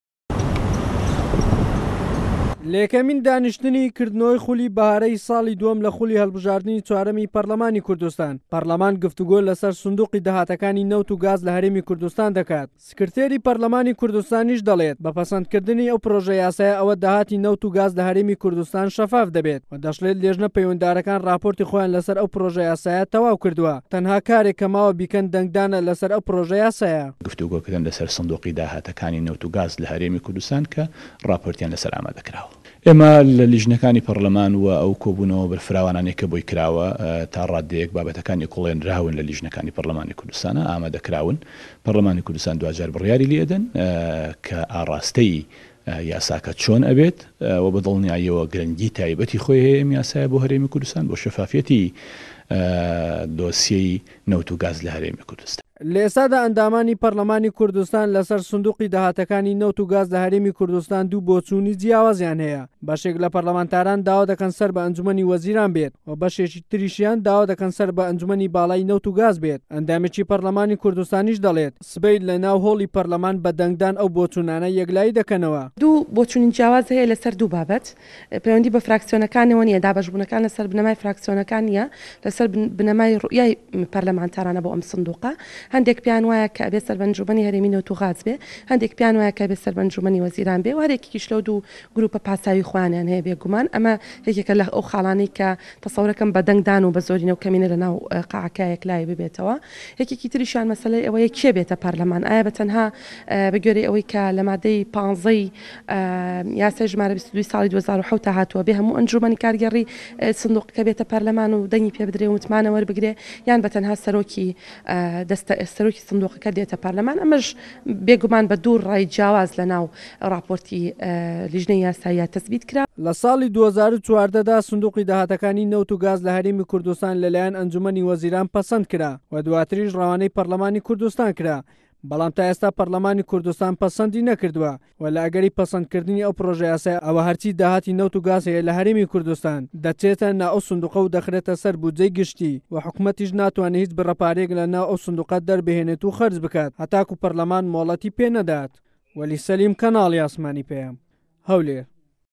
سبه‌ینێ په‌رله‌مان كوردستان كۆبوونه‌وه‌كانی ده‌ستپێده‌كاته‌وه‌
پڕۆژه‌یاسای سندوقی داهاتی نه‌وت‌و غازی هه‌رێم تاووتوێده‌كات
ئه‌بوبه‌كر هه‌ڵه‌دنی
ئه‌ندامی په‌رله‌مانی كوردستان
شێروان زرار
وته‌بێژی كۆمسیۆنی باڵای هه‌ڵبژاردن‌و راپرسیی هه‌رێم